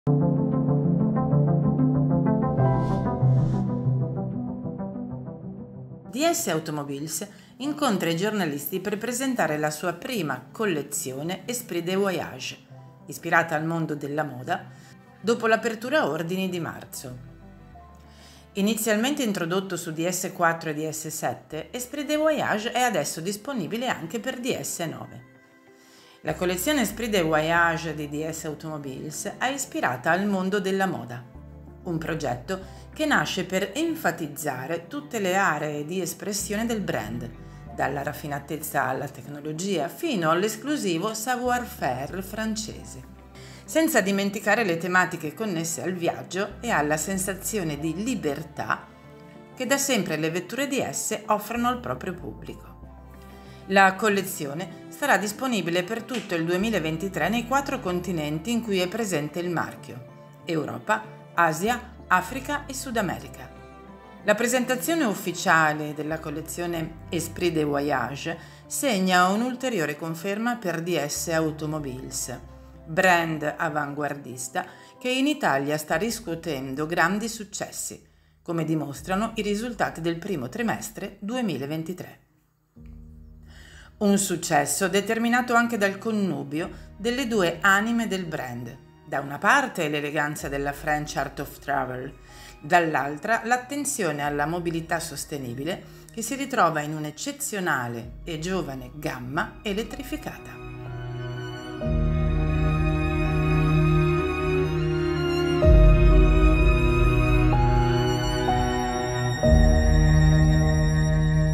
Presentata la collezione Esprit de Voyage - Motor News n° 17 (2023)
DS Automobiles incontra i giornalisti per presentare la sua prima collezione Esprit de Voyage, ispirata al mondo della moda, dopo l’apertura ordini di marzo. Inizialmente introdotto su DS 4 e DS 7, Esprit de Voyage è adesso disponibile anche per DS 9.
Servizio con la voce di Lilia Marcucci.